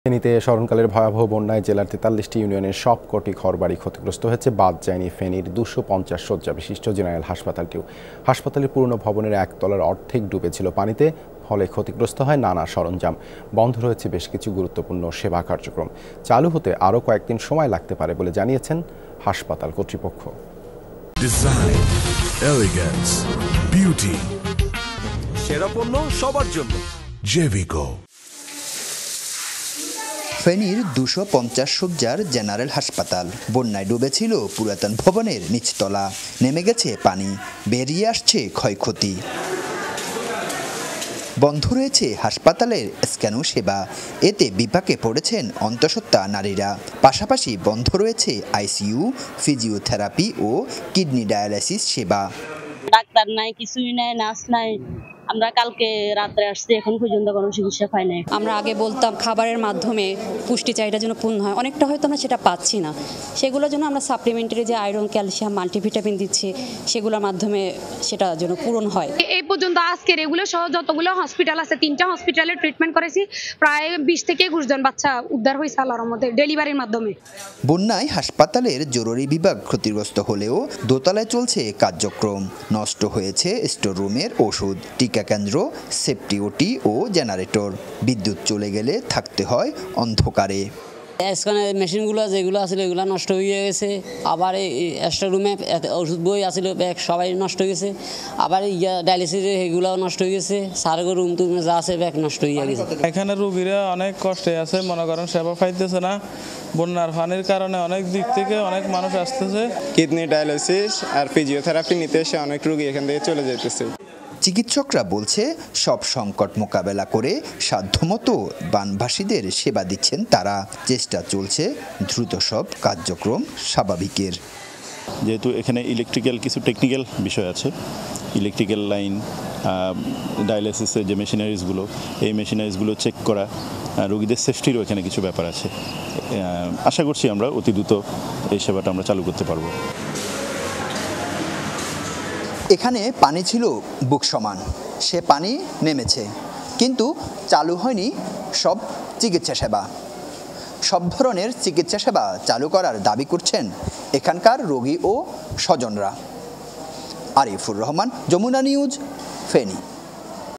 বন্যায় হাসপাতালে ক্ষতিগ্রস্ত নানান সরঞ্জাম; ব্যাহত সেবা কার্যক্রম | Feni Hospital | Jamuna TV
ফেনীতে স্মরণকালের ভয়াবহ বন্যায় জেলার ৪৩টি ইউনিয়নের সবকটি ঘর-বাড়ি ক্ষতিগ্রস্ত হয়েছে। বাদ যায়নি ফেনীর ২৫০ শয্যা বিশিষ্ট জেনারেল হাসপাতালটিও। হাসপাতালের পুরনো ভবনের এক তলার অর্ধেক ডুবে ছিলো পানিতে। ফলে ক্ষতিগ্রস্ত হয় নানান সরাঞ্জাম। বন্ধ রয়েছে বেশ কিছু গ্রুরুত্বপূর্ণ সেবা কার্য্ক্রম। চালু হতে আরও কয়েকদিন সময় লাগতে পারে বলে জানিয়েছেন হাসপাতাল কর্তৃপক্ষ।

বন্যায় হাসপাতালে ক্ষতিগ্রস্ত নানান সরাঞ্জাম; ব্যাহত সেবা কার্যক্রম | Feni Hospital | Jamuna TV

Address:
Bangladesh.